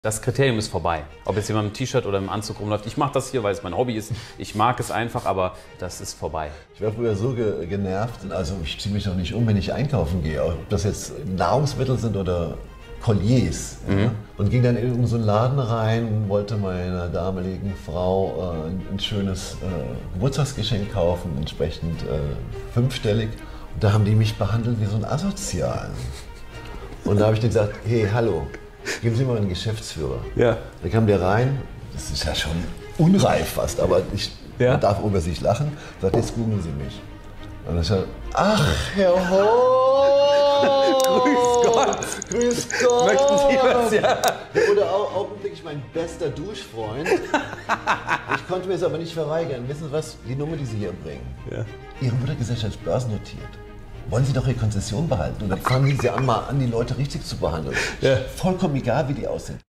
Marc Gebauer im Interview
»Ach, Herr Hoooomm.«
Offensichtlich entsprach mein damaliges Erscheinungsbild nicht den Vorstellungen der Angestellten eines Juweliers.
Als ich dann nach dem Geschäftsführer fragte und bat, dass dieser meinen Namen googeln sollte, wurde er augenblicklich zu meinem besten Duzfreund – das war vor allem dem Umstand geschuldet, dass ich sehr liquide war.
,,Ihr Mutterunternehmen ist börsennotiert. Wollen Sie Ihre Konzession noch behalten? Dann fangen Sie mal an, die Leute richtig zu behandeln – vollkommen egal wie die aussehen’’.
Das konnte ich mir nicht verkneifen. Die Situation wirkt aus heutiger Sicht fast unreif, aber man darf auch über sich selbst lachen.